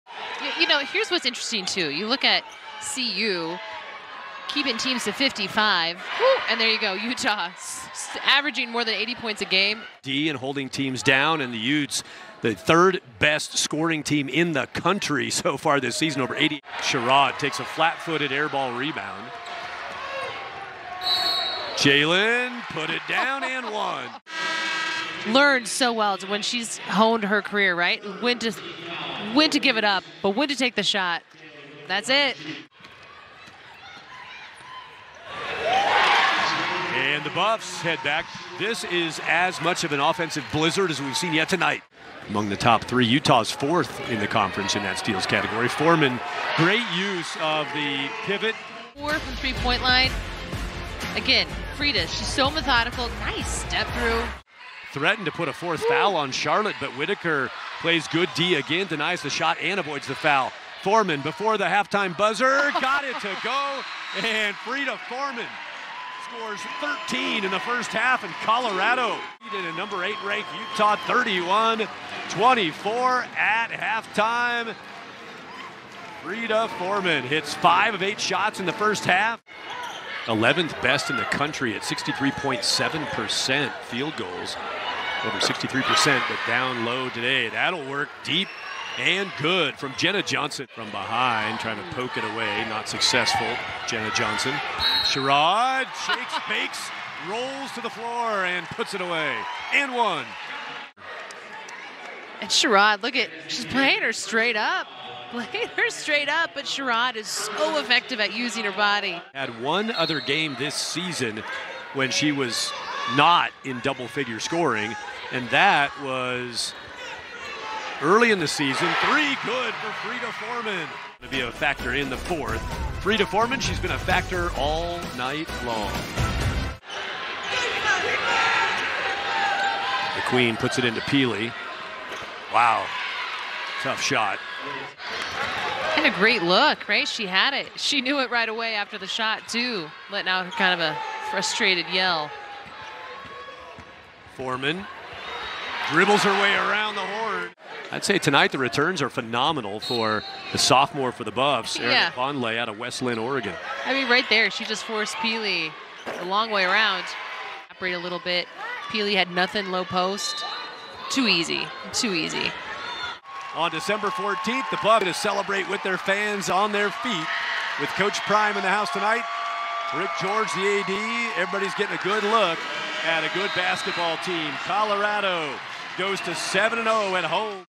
No. 8 Utah vs. Colorado | Game Highlights | Women's College Basketball | 2022-23 Season
Colorado women's basketball earned a 77-67 win over No. 8 Utah, handing the Utes their first loss of the season. Frida Formann scored a game-high 20 points. This is the Buffs first win against a top-10 opponent since beating Stanford in 2021.